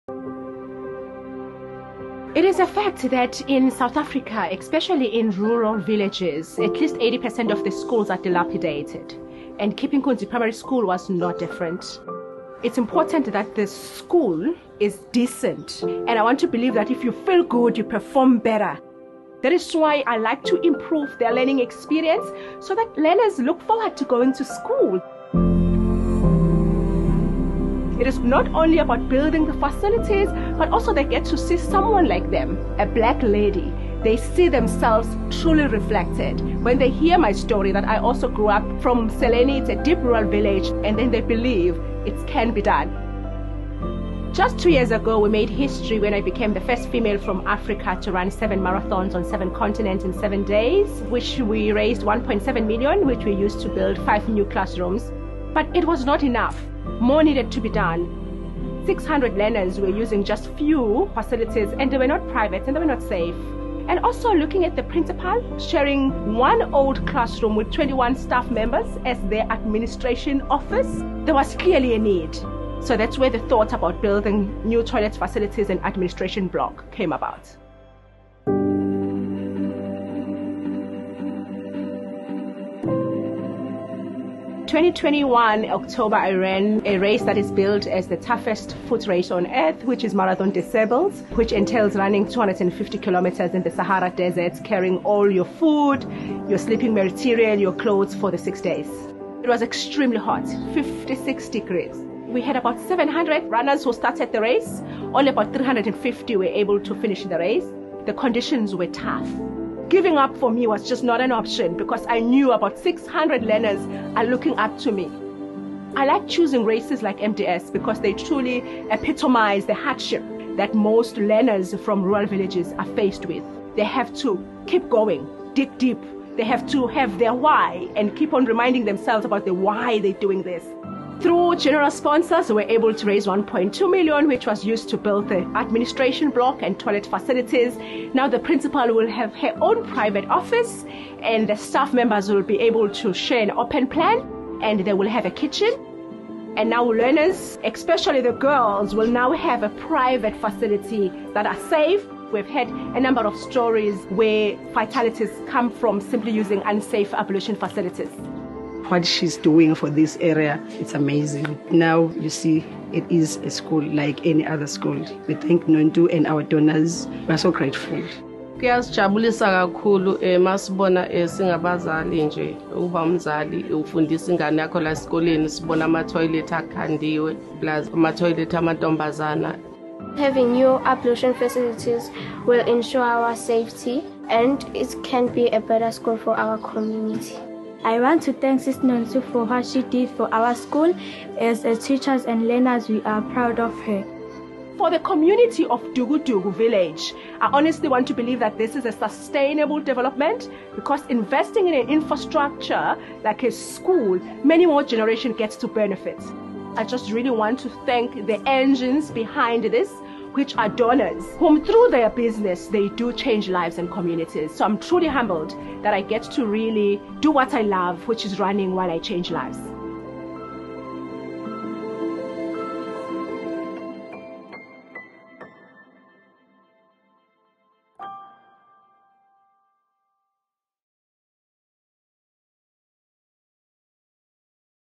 Go Beyond for A Child
On 19 September Nontuthuko Mgabhi will be running 220km in the Bolivian Altiplano to raise funds for 220 school shoes and children’s books. Be part of this initiative by donating a school shoe to a child in need. This initiative aims to give children from deep rural villages a fair chance for education.